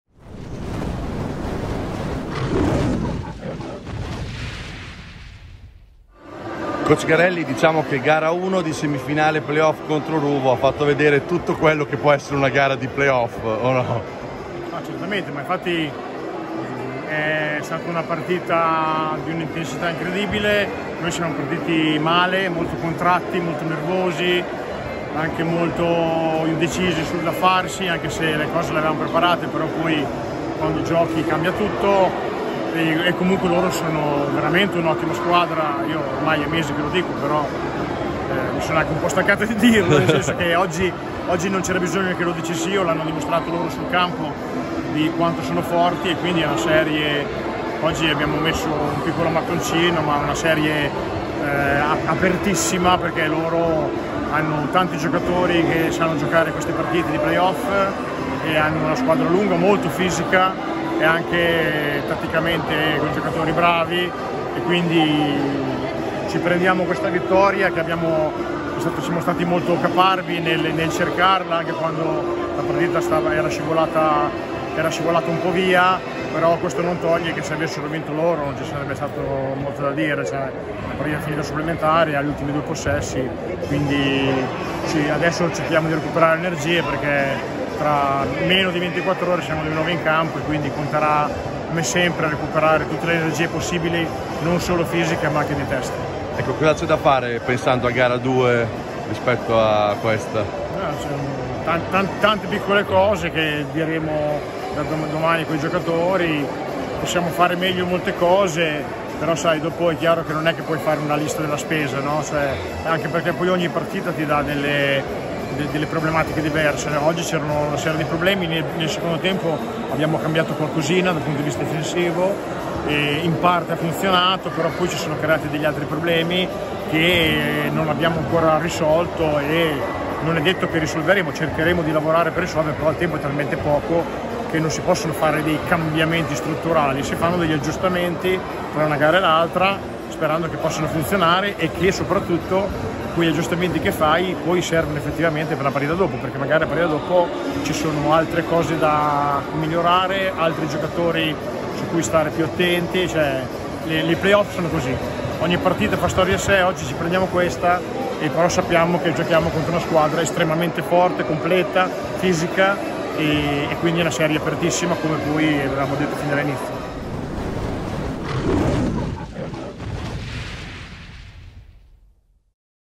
Gara 1 Semifinale Play Off Blacks Faenza - Ruvo di Puglia 77-73 d1ts il post gara di coach Garelli
L'allenatore faentino commenta la vittoria dei Raggisolaris